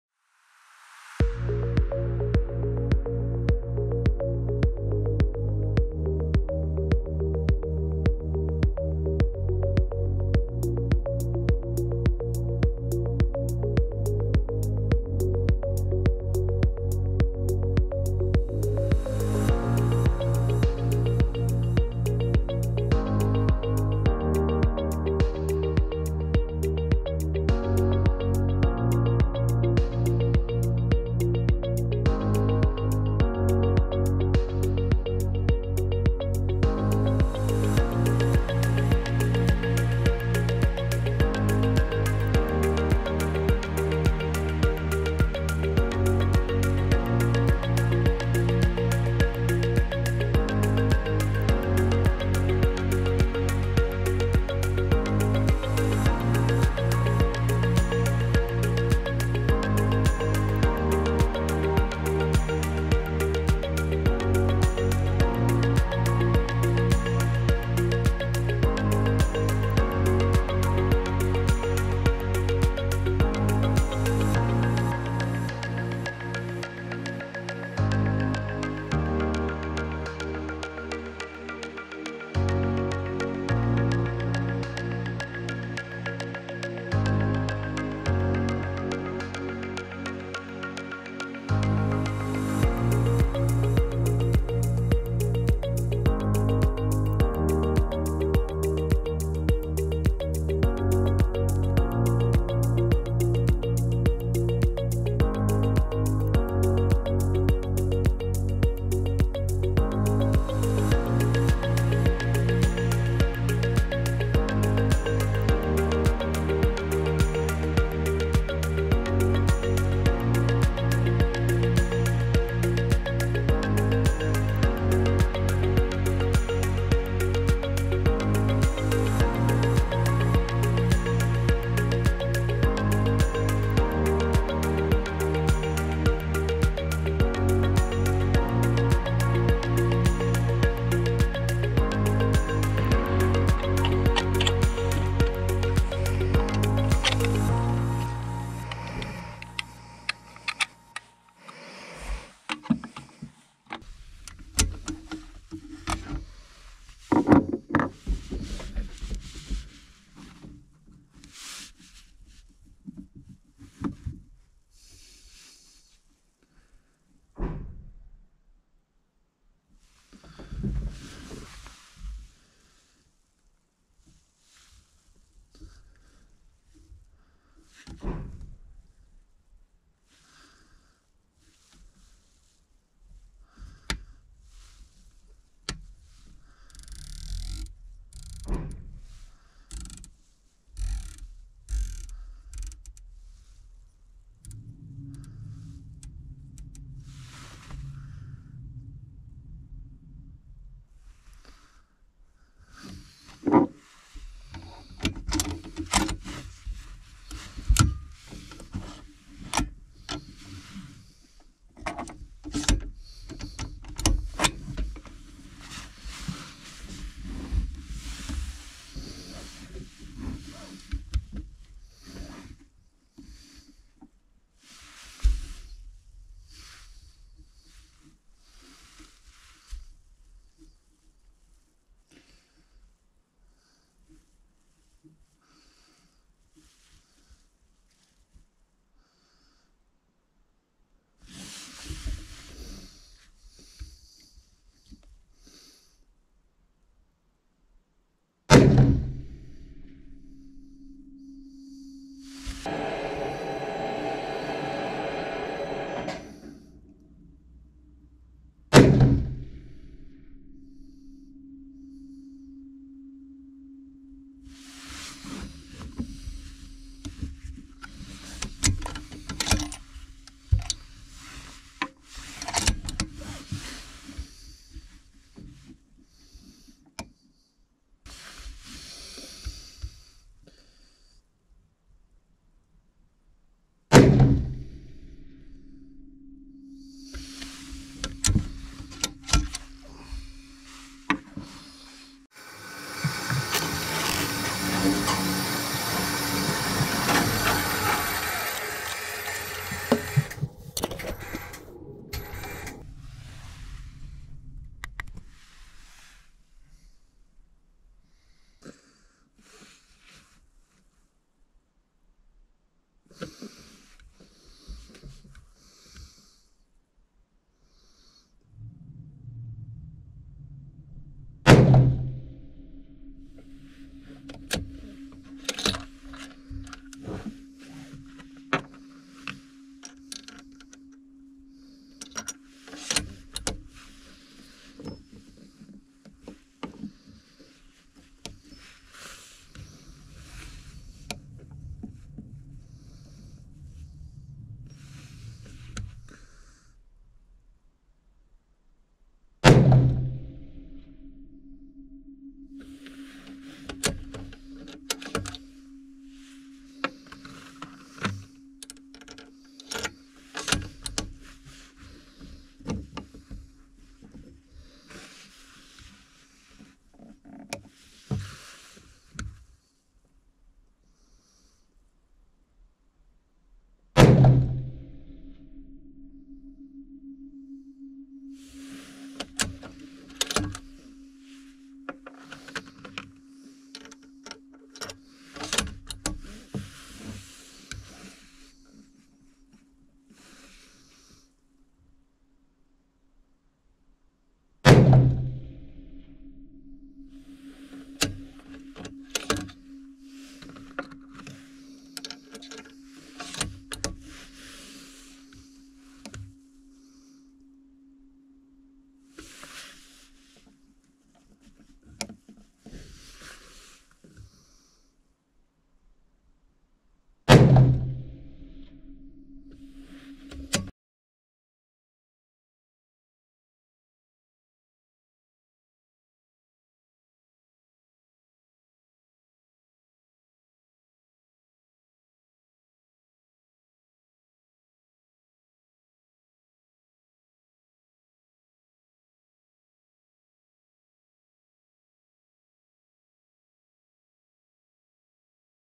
Serie - We shoot them all - Tikka T3x TAC A1 Coyote - .308 Winchster
Setting ab a new Tikka T3x TAC A1 Coyote with a ERATAC 20MOA Blockmount and Vortex Strike Eagle 5-25x56mm FFP MRAD
Kaliber: - .308 Winchster
Testrange - 100m-300m
Ammo: GGG .308Win- HPBT 175gr

Thanks to the company Lubet for providing the weapon.